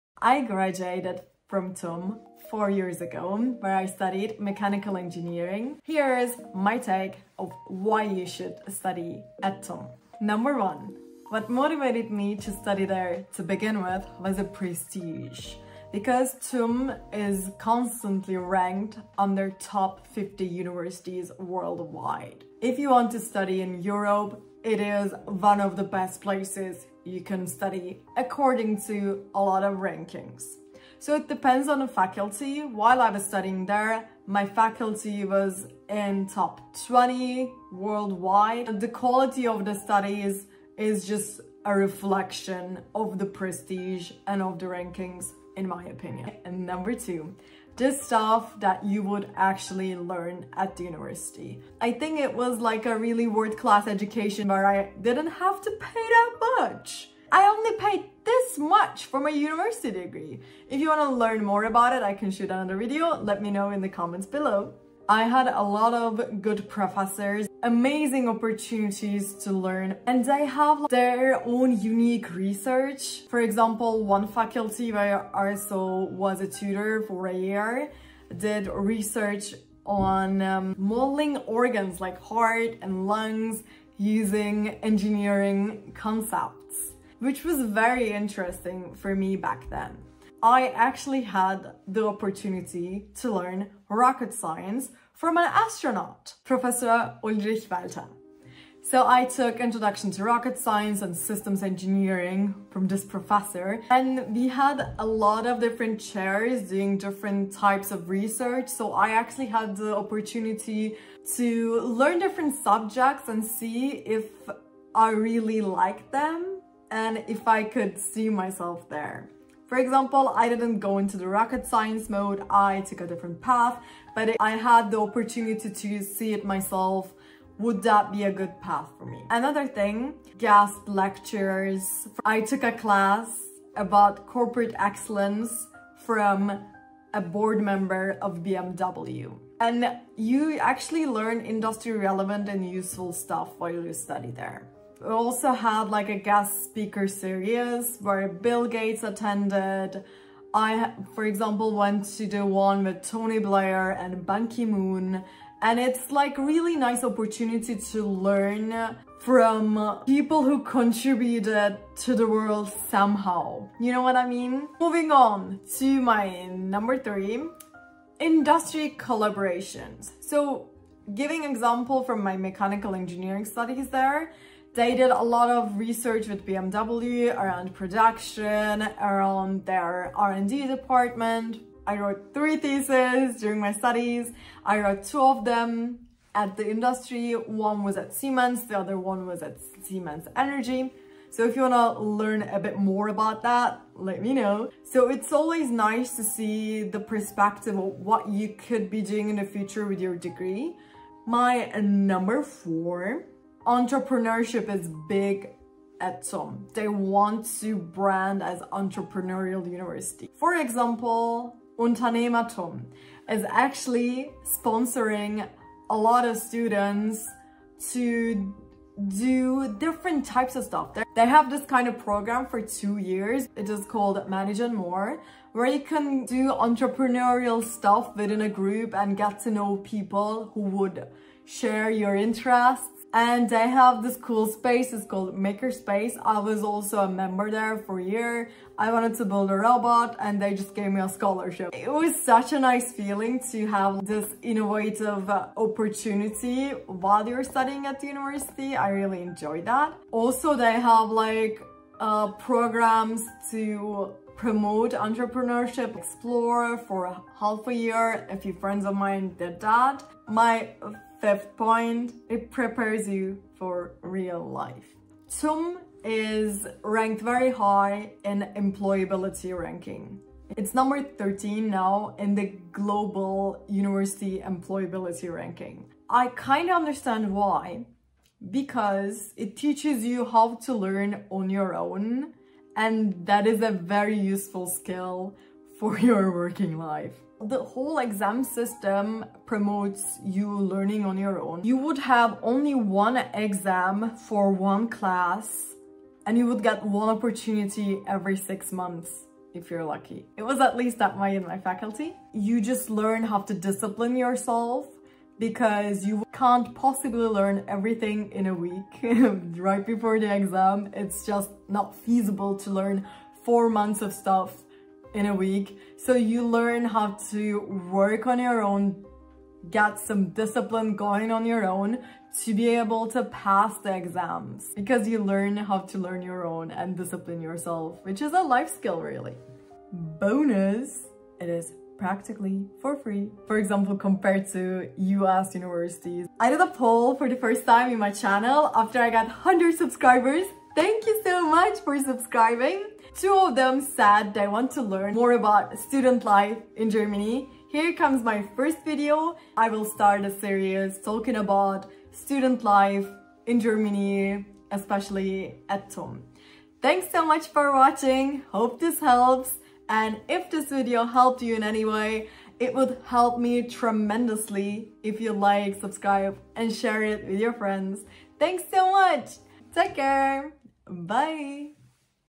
5 Reasons to Study at TUM (Technical University of Munich)
Hi all, I hope this information helps you! Here is a break-down of the video:

5 reasons to study at the Technical University of Munich
1. Prestige: Great rankings for TUM are there for a reason.
2. Learning opportunities: Great professors and interesting subjects. Guest lectures and speaker series and many more things to learn! For example, I had the opportunity to learn rocket science and systems engineering from an actual astronaut. How cool is that?
3. Industry collaborations: E.g.  a lot of collaborations with BMW at the Mechanical Engineering Faculty. I had the opportunity to write one thesis at Siemens and another one Siemens Energy.
4. Entrepreneurship: Unternehmer TUM, MakerSpace, Xplore and many more programs.
5. Real life aspect: TUM is ranked very high in employability rankings.